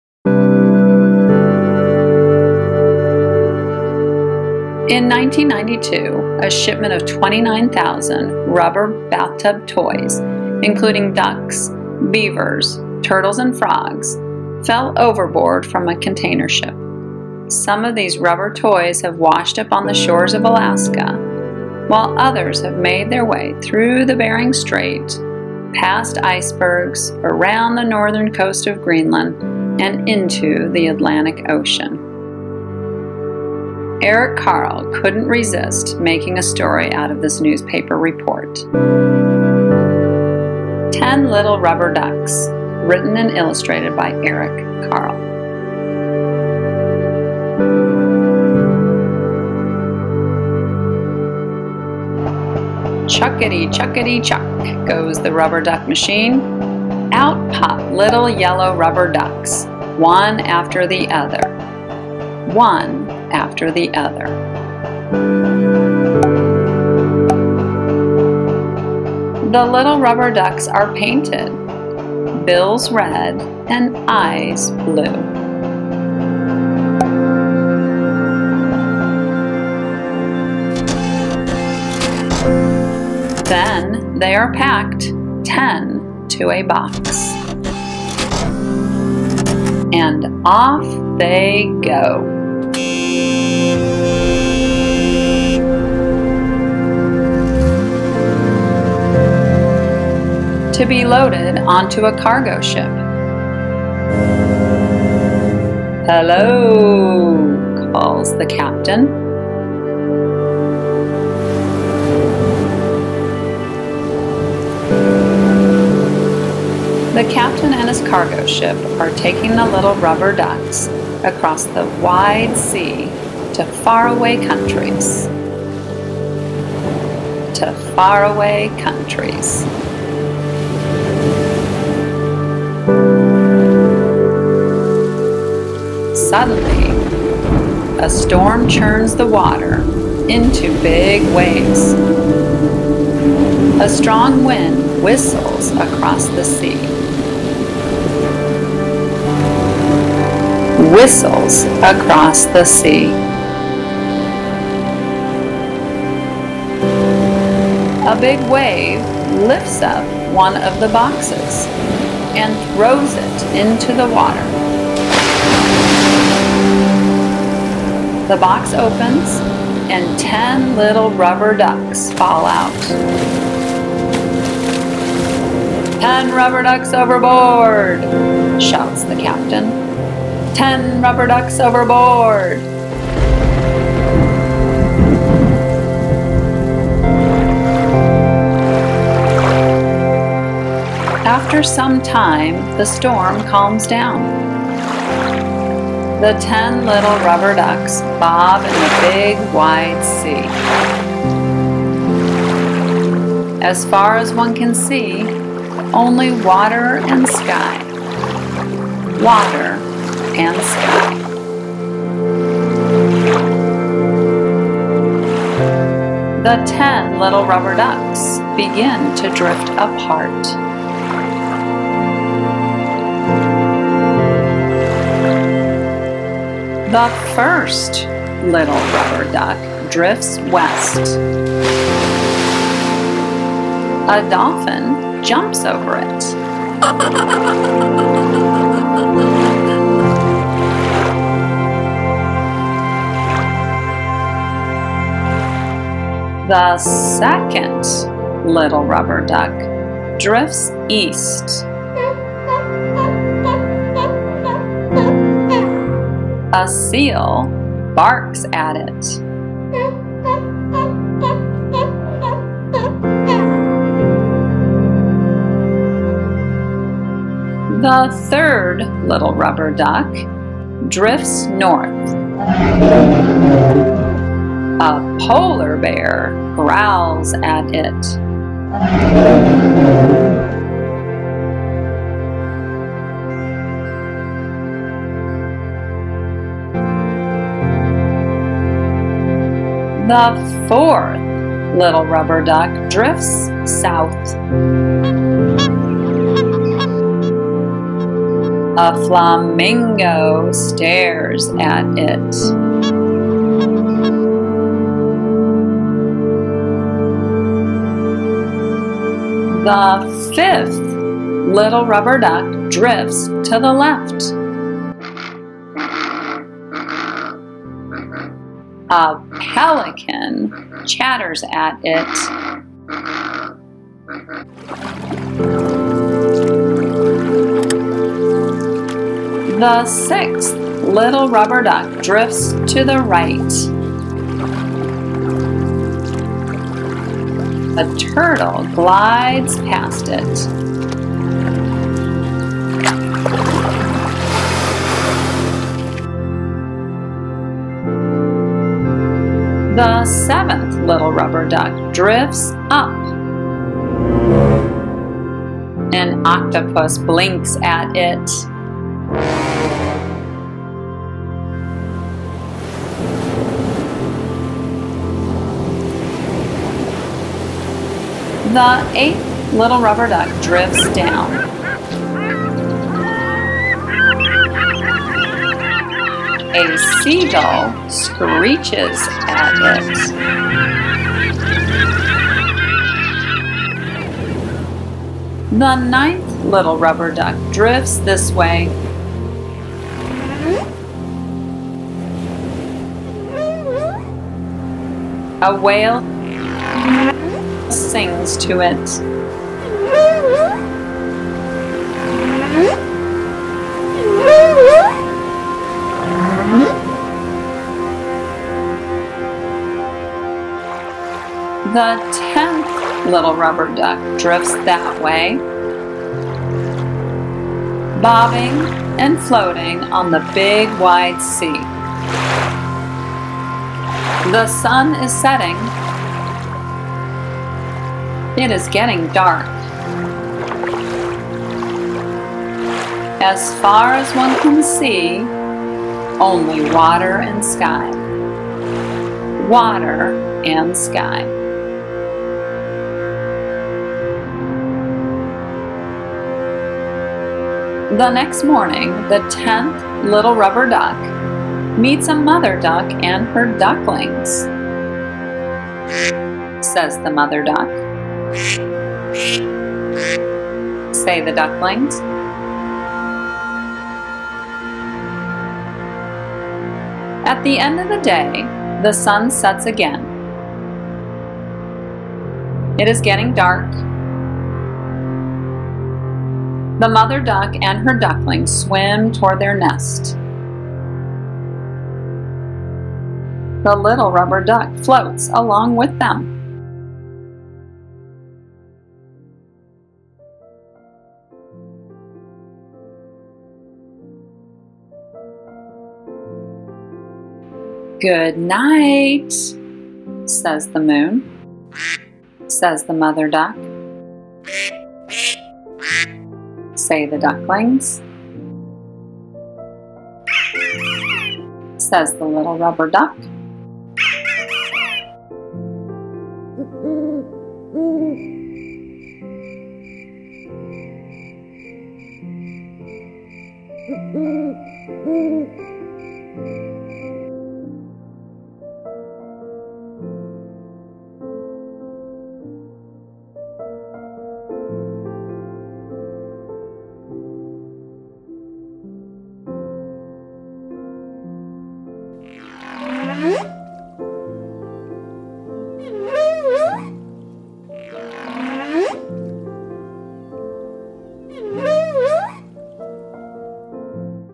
10 Little Rubber Ducks w/ Words, EFX & Music
Eric Carle's fun tale about 10 rubber ducks adrift in the ocean after falling off a container ship.   Inspired by a true story.